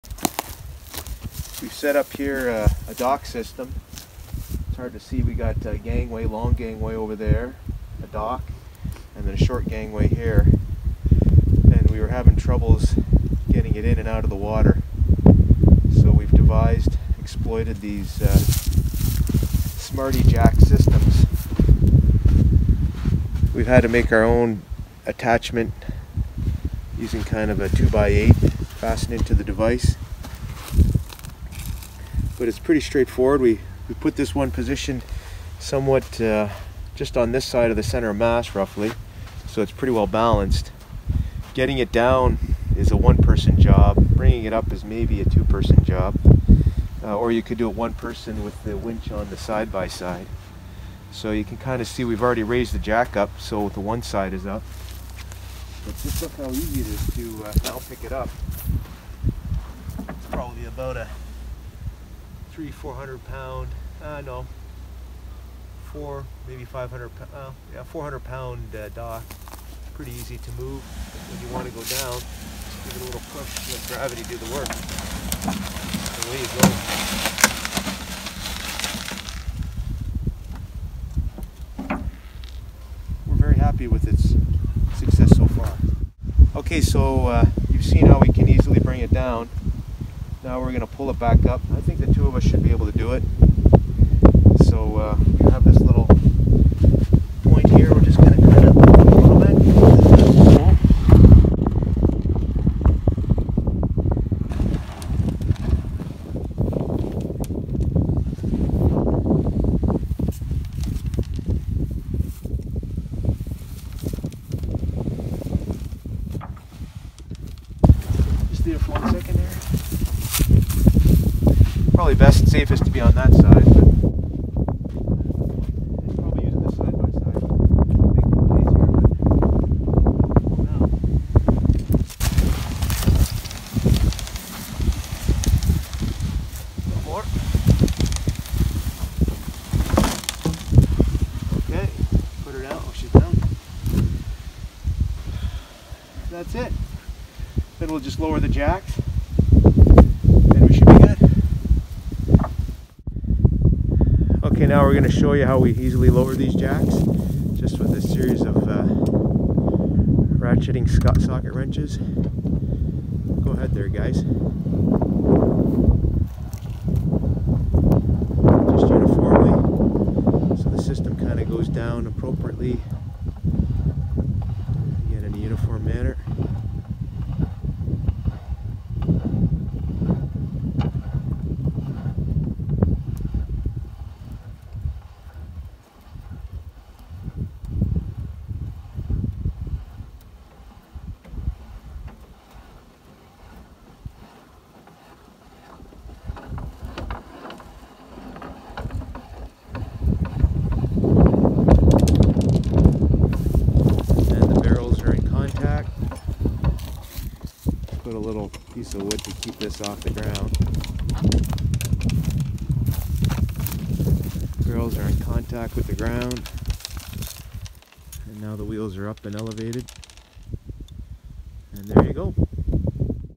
Making a Movable Dock with the Smarte Jack
The Smarte Jack is meant to be used as an easy way to move and level boat lifts. In this video, you'll see how we engineered the Smarte Jack to help make moving our dock and gangways out of the water a whole lot easier.